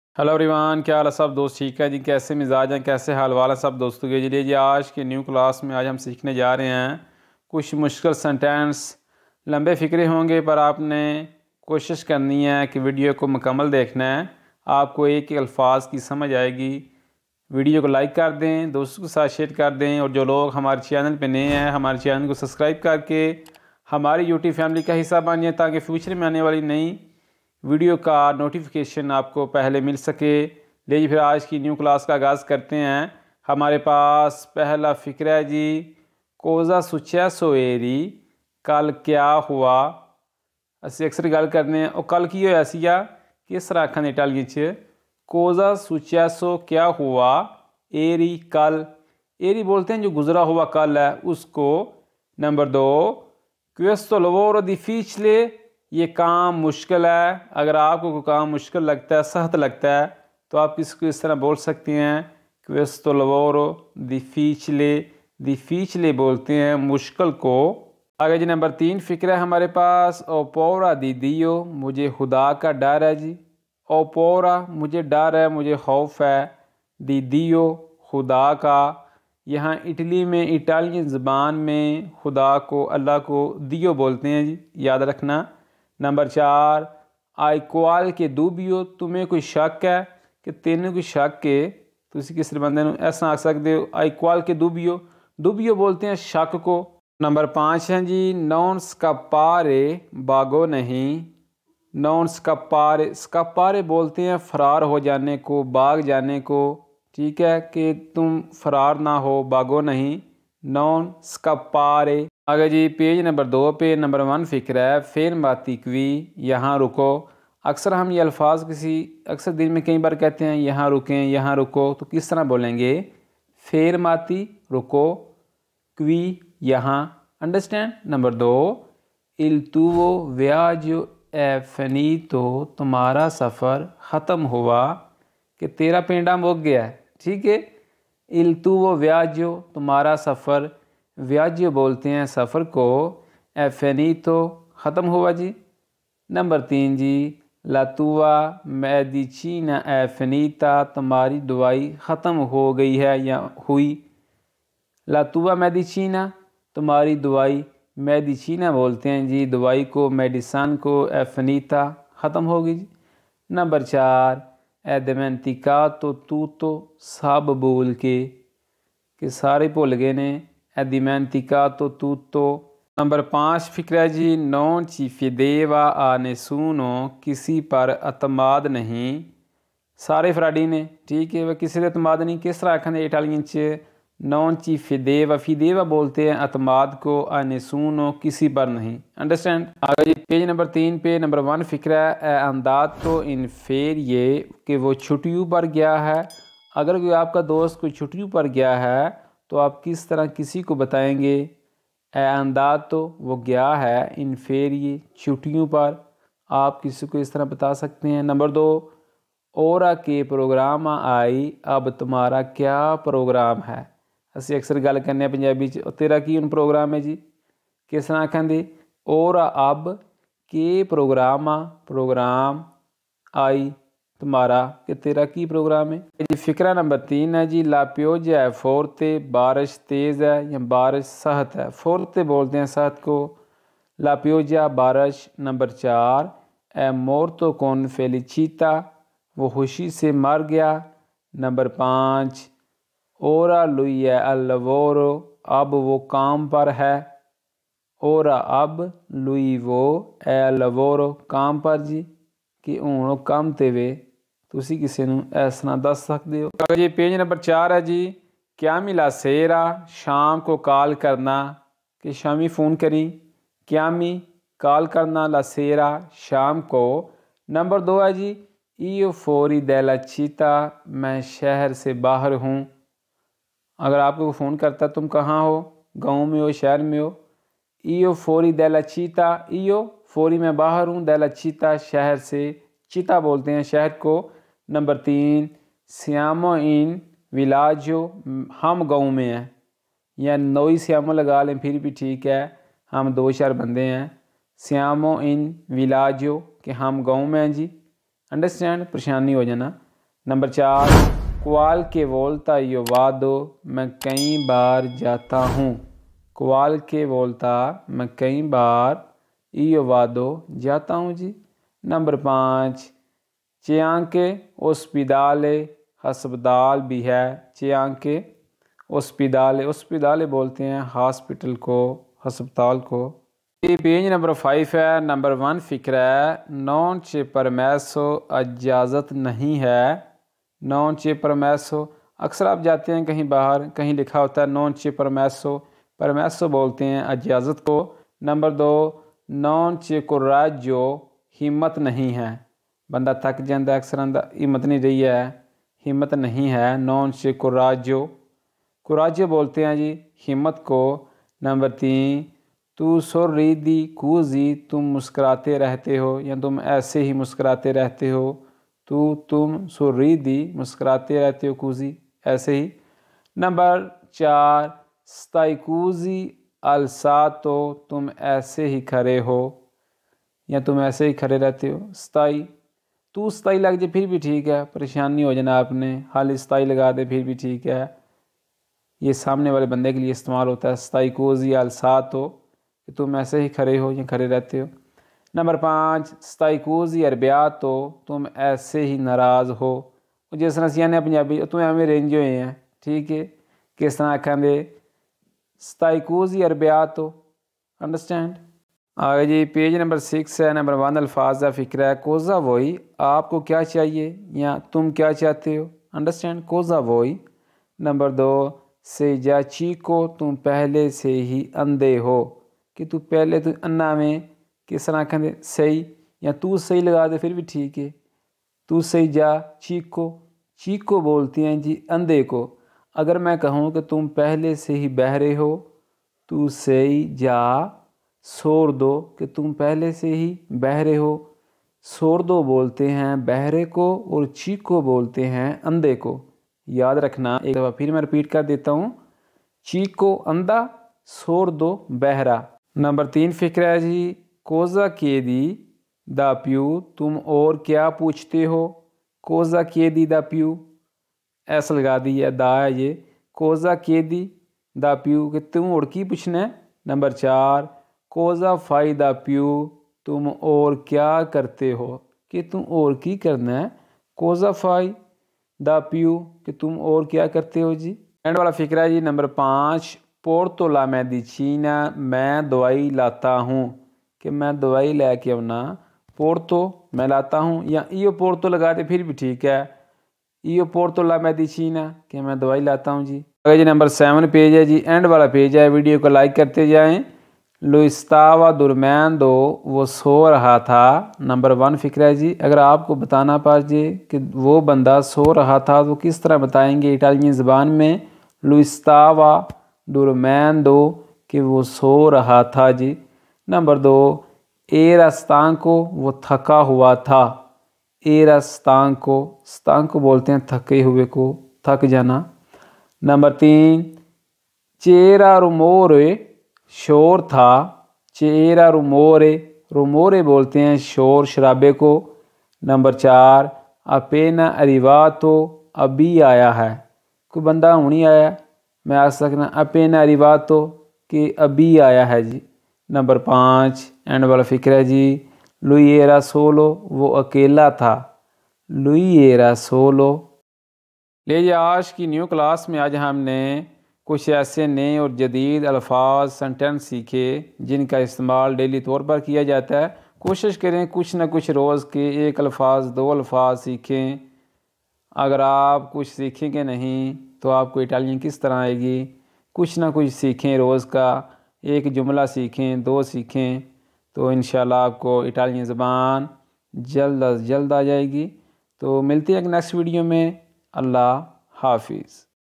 35 Italian Sentences for Daily Use with Urdu Translation for Beginners | Speaking practice with Ali
35Italian Sentences for Daily Use with Urdu Translation for Beginners |Speaking practice with Ali